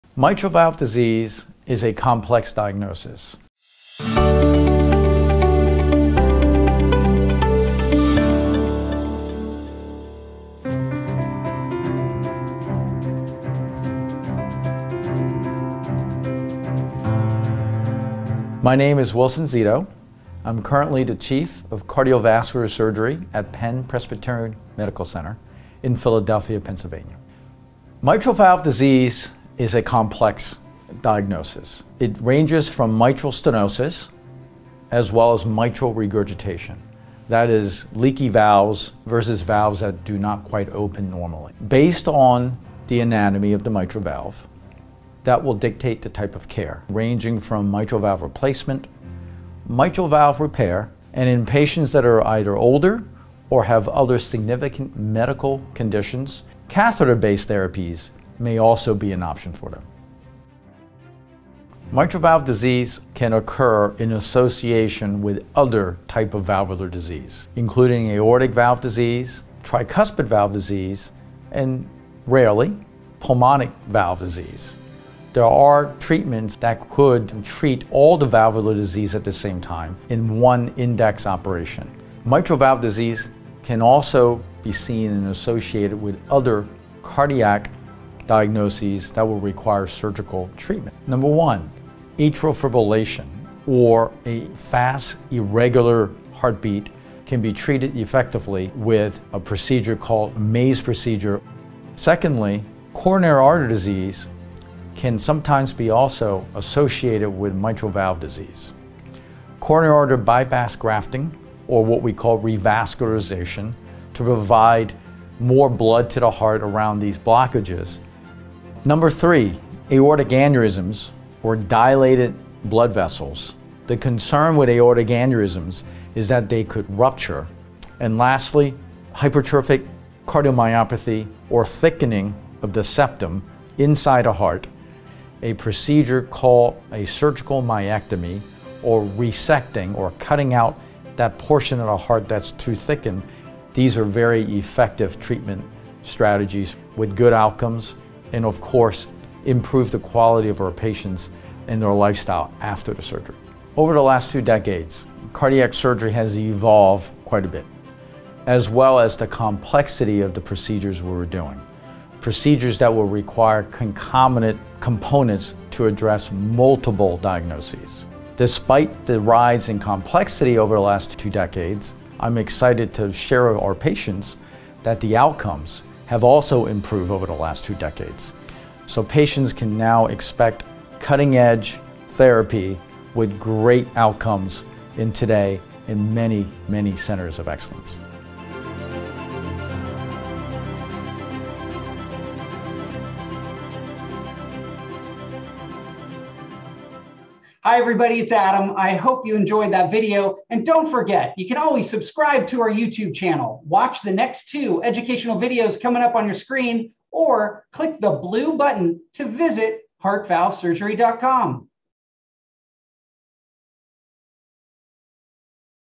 Mitral Valve Awareness: Complex Mitral Valve Surgery with Dr. Wilson Szeto
- Mitral valve disease is complex.  In fact, mitral valve disease is often associated with several different types of heart disease including atrial fibrillation, coronary artery disease, aortic aneurysms and hypertrophic cardiomyopathy.

In this video, Dr. Wilson Szeto, Chief of Cardiovascular Surgery at Penn Presbyterian Medical Center in Philadelphia, Pennsylvania, explains how advanced surgical techniques are used during "concomitant procedures" to treat mitral valve disease and related cardiac conditions during one operation.

Key questions answered by Dr. Szeto in this video include:

1. Why is mitral valve disease considered a complex form of heart disease?

2. Is it common for mitral valve disease patients to have  other forms of valvular disorders in the aortic, tricuspid and pulmonary valves?

3. What is the relationship among mitral valve disease and cardiac conditions including coronary artery disease, atrial fibrillation, hypertrophic cardiomyopathy and aortic aneurysms?

4. How can surgeons use advanced surgical techniques to treat multiple cardiac conditions during a single "concomitant procedure"?

5. What should patients know about the outcomes of complex procedures used to fix mitral valve disease and other forms of cardiac disease?